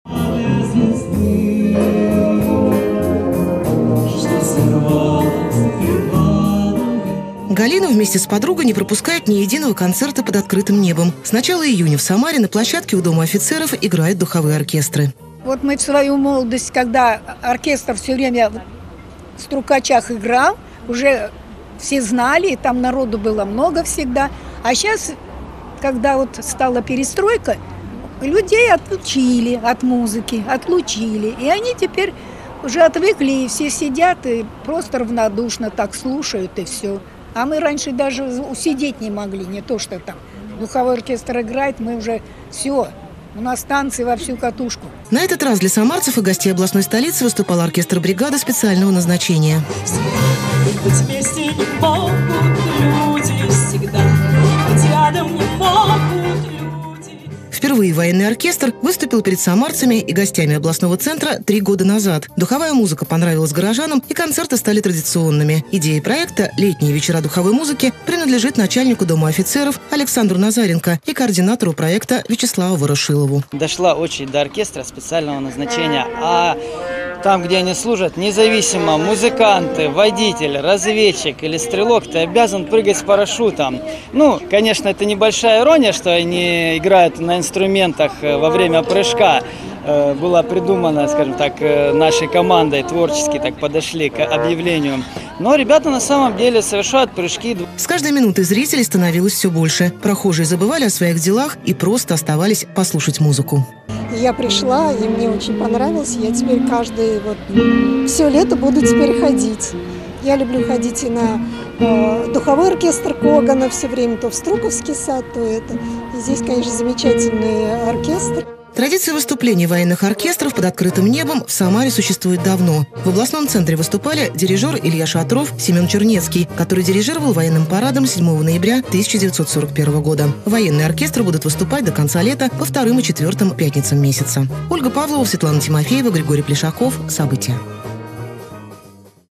В Самаре проходят "Летние вечера духовой музыки"
В Самаре продолжается традиция концертов под открытым небом. На площадке у Дома офицеров играют военные духовые оркестры. На этот раз перед горожанами и гостями областного центра выступал оркестр бригады специального назначения. Подробности у корреспондентов "Событий".